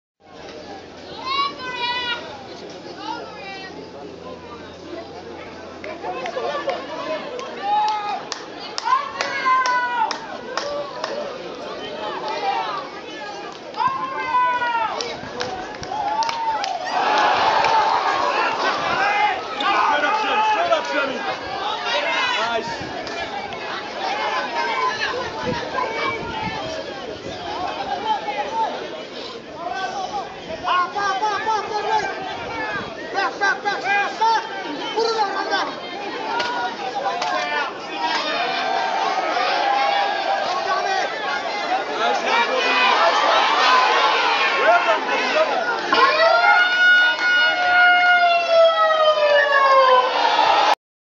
Digicel Cup 2013 - Gurias v Vipers
Digicel Cup 2013 - Gurias v Vipers at the Lloyd Robson Oval Port Moresby. Courtesy of